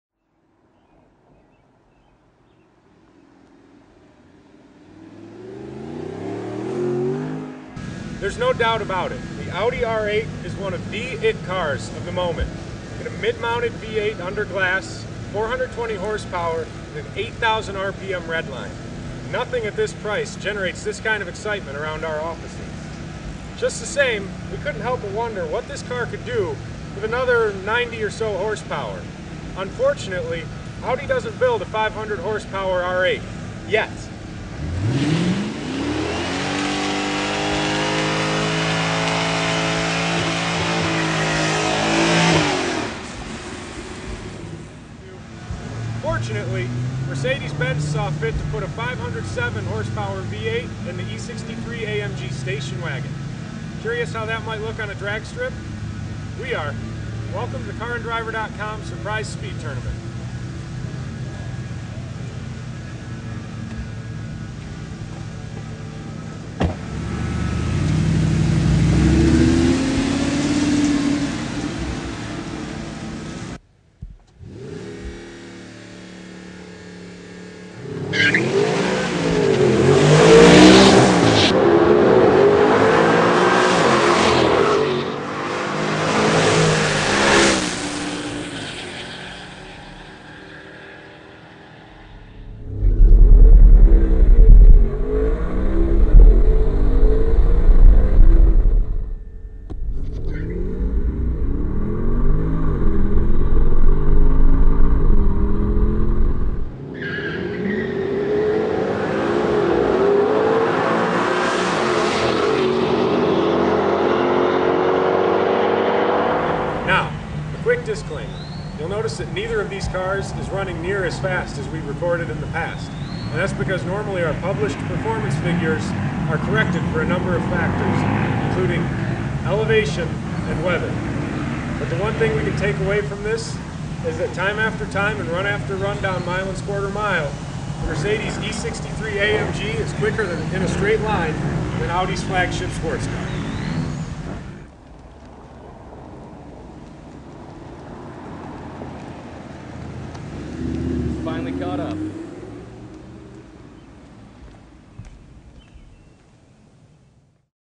Station Wagon vs Supercar! Audi R8 vs Mercedes-Benz E63 AMG
The Mercedes-Benz E63 AMG wagon takes on the Audi flaghip R8 exotic sports car in the 1/4 mile drag race. Guess who wins...